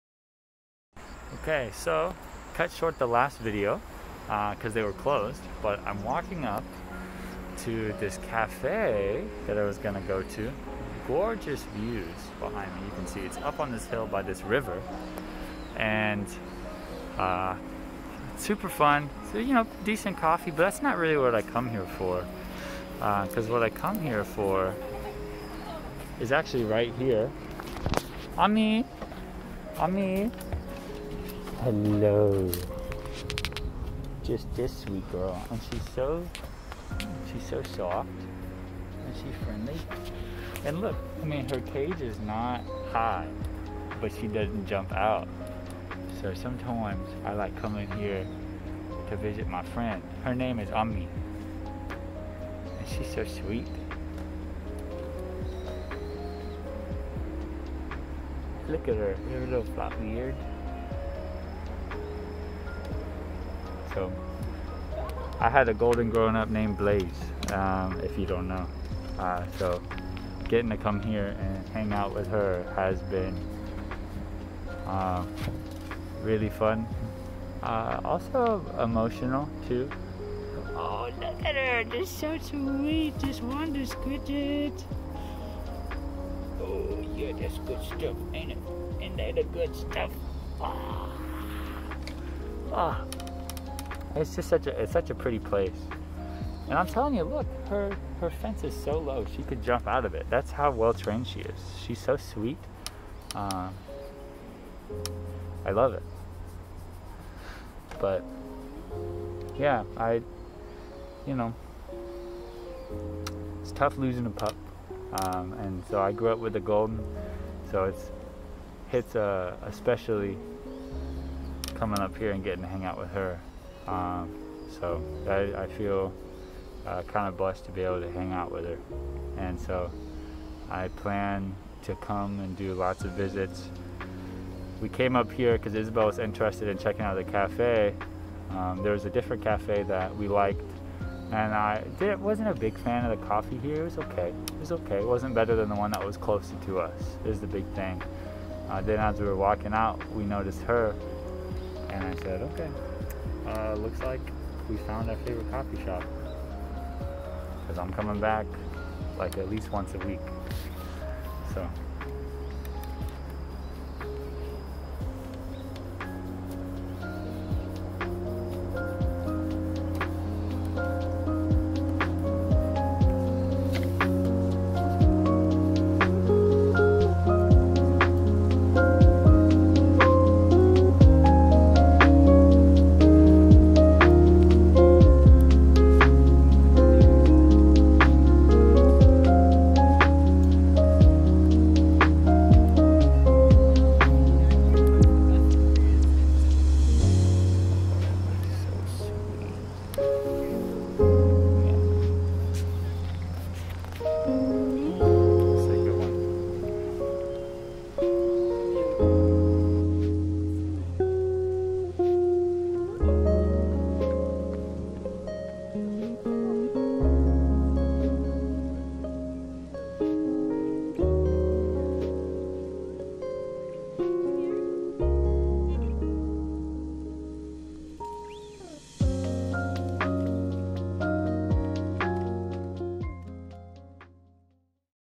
My Korean best friend
Man I love that pup :)

Moved to South Korea with my wife to teach English!

-Zee